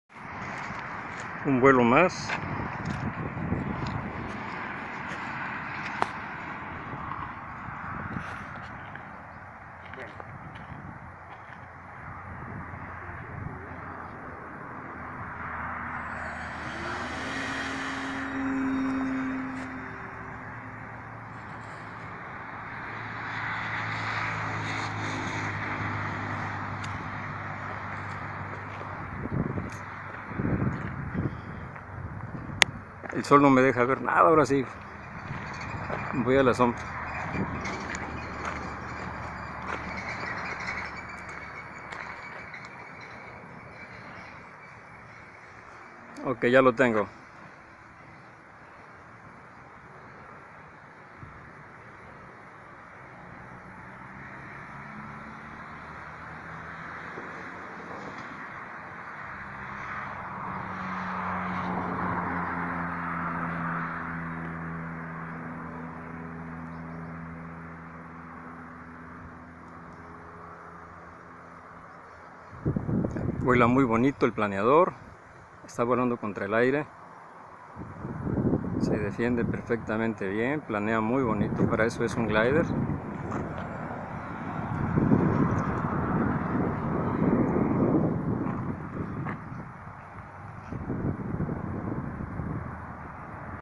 VUELO DE PLANEADOR DE 2 METROS EN VERACRUZ HECHO EN CASA
VUELO DE PLANEADOR DE 2 METROS DE ENVERGADURA  REALIZADO EN ACAYUCAN VER.
EXCELENTE GLIDER HECHO A MANO EN FOAM BOARD DE 5 mm
envergadura de 204  cm CON LONGITUD DE  1,220 mm  volando
 como un avion de diseño,  O MEJOR,  MOTOR  2216  DE 1400 KV  RACER STAR, PILA  11.1
 5200  MAH,  SERVOS DE 9 GRS.  HELICE  11X7,  ESC  ECONOMICA